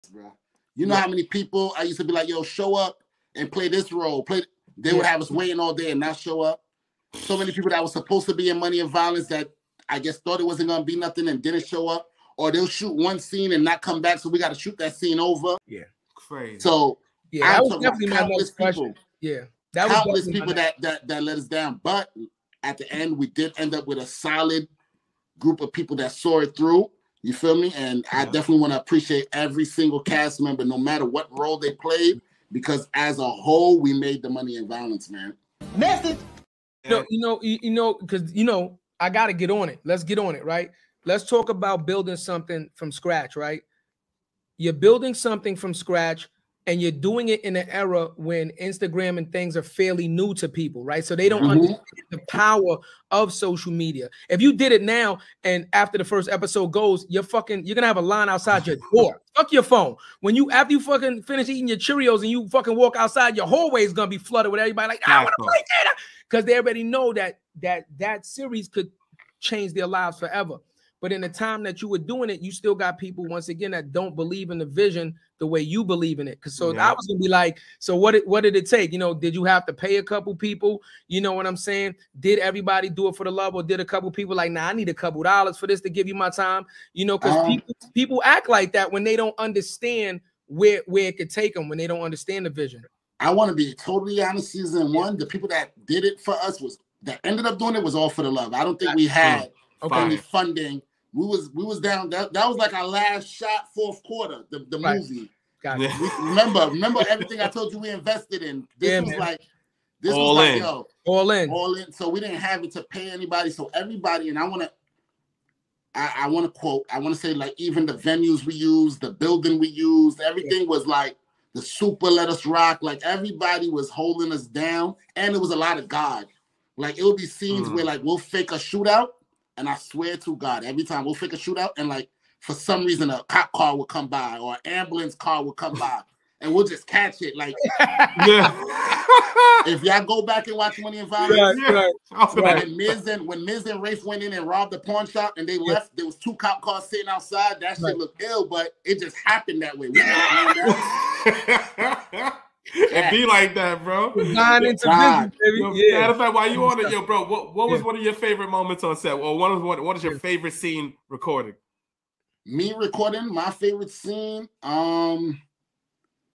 The Casting Struggle: How 'Money & Violence' Triumphed
Against All Odds: Learn How Ace General Launched A Hit Show With Zero Budget
Think making your own hit show requires big money and resources? Ace General proves otherwise. With pure hustle, grit and a whole lot of heart, Ace transformed his zero-budget passion project into Money and Violence - one of social media's most buzzed about series.

Join Ace and More Than A Title hosts Jarrett and Chadeo as he shares the untold story of Money and Violence' grassroots rise. Learn how he motivated a team to keep filming without pay, maneuvered past obstacle after obstacle, and leveraged the power of authenticity to attract millions of viewers.

Whether you want to turn your creative vision into reality or simply need inspiration to follow your dreams, Ace's incredible underdog triumph will give you the faith and fire to build something from nothing.